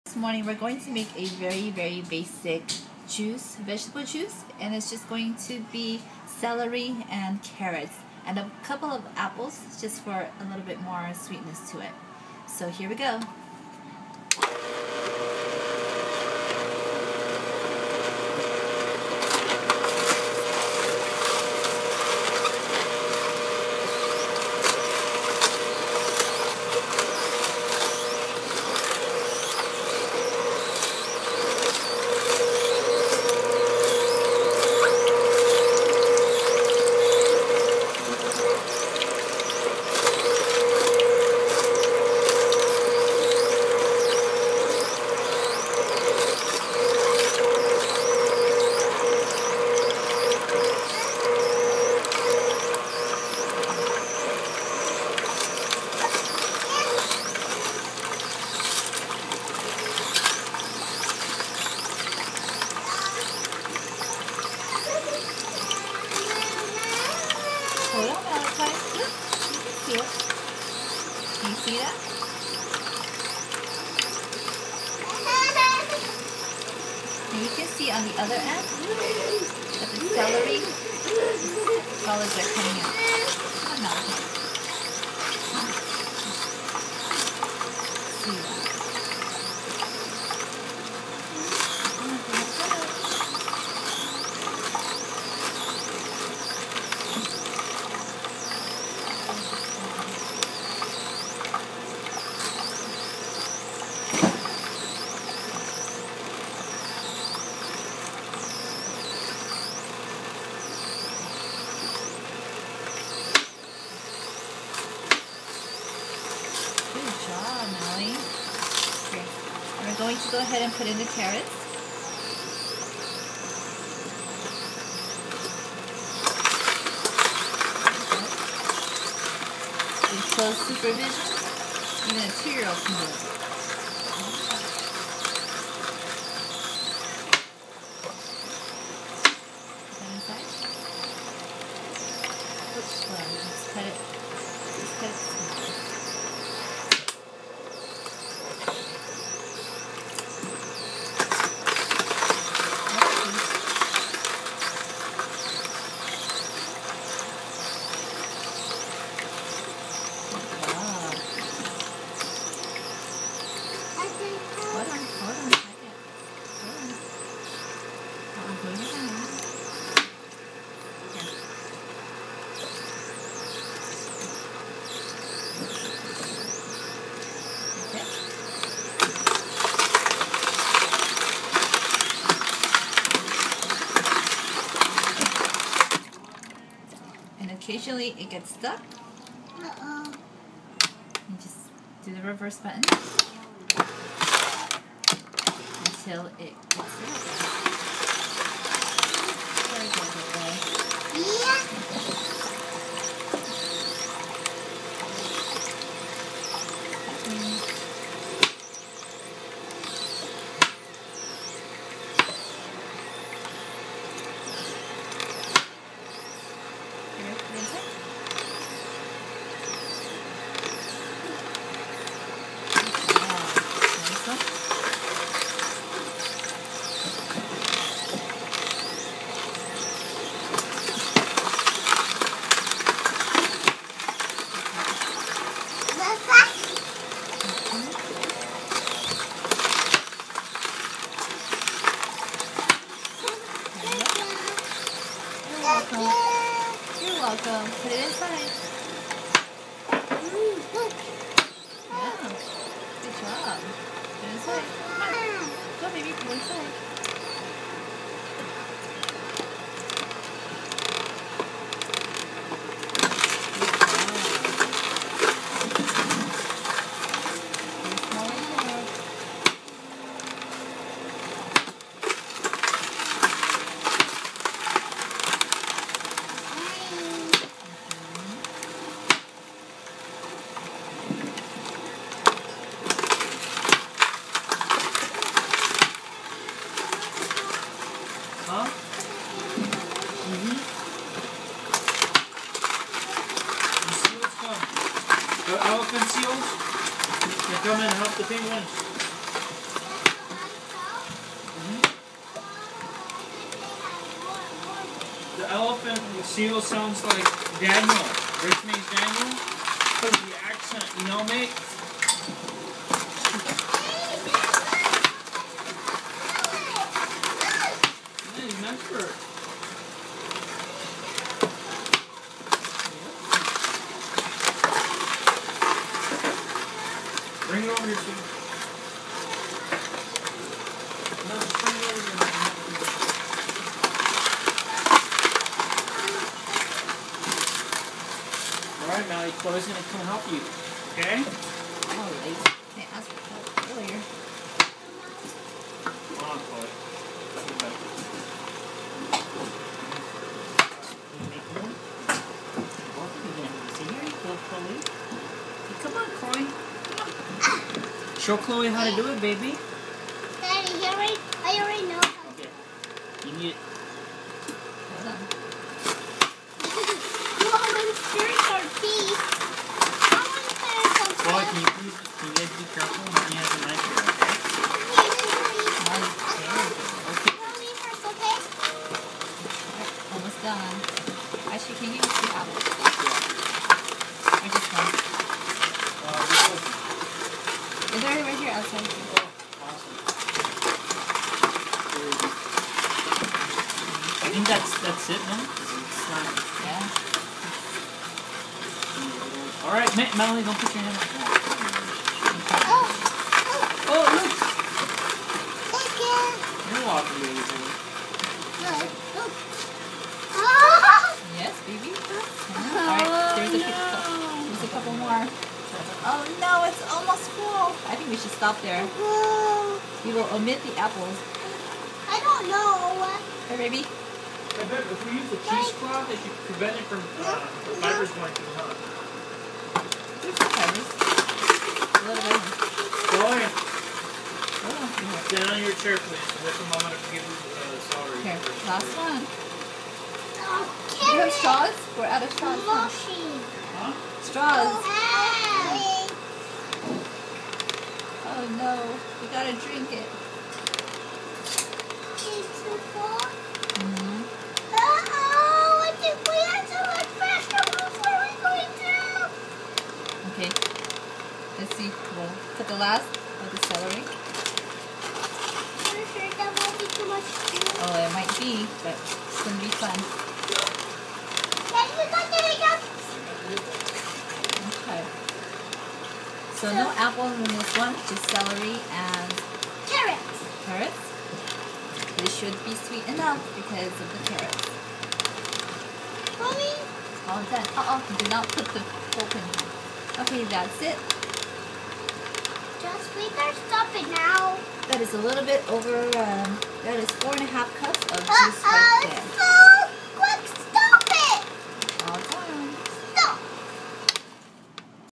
Juicing Carrots and Celery Omega VRT 350
Easy way to get some veggies in your daily consumption and it tastes good. No better reason to juice other than better health :) omega vert 350 is the juicer you see here.